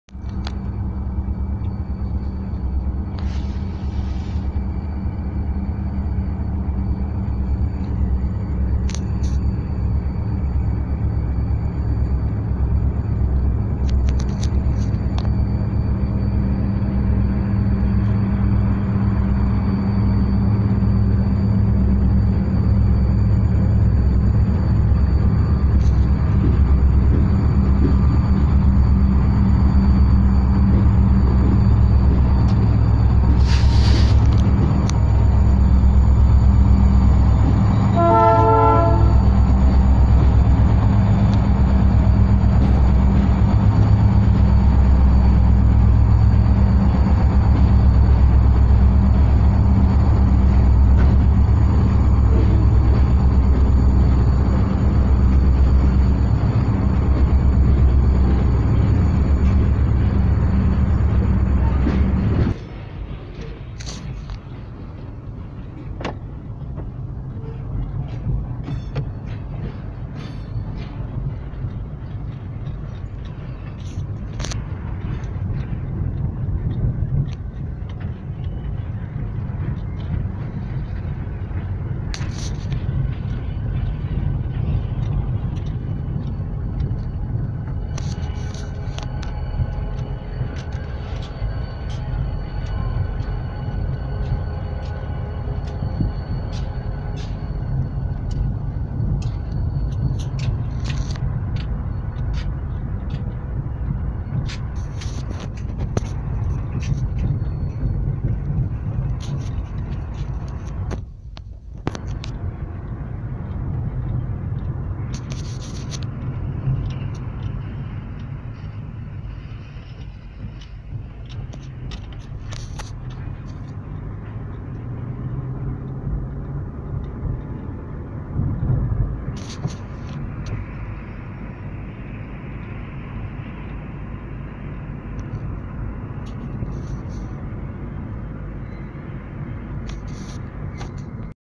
Tren ferromex 3152 3148
Este tren lo vi en san Nicolás con el tren mixto Rumbo a patios de ferromex saludando con el silbato la #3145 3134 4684 están esperando a que le den su tren para irse  tenía vagones tolva , de carbon y de platas de pemex eran unas SD40-2 Grabado lunes 18 febrero 2019 a las 5:02 pm saludos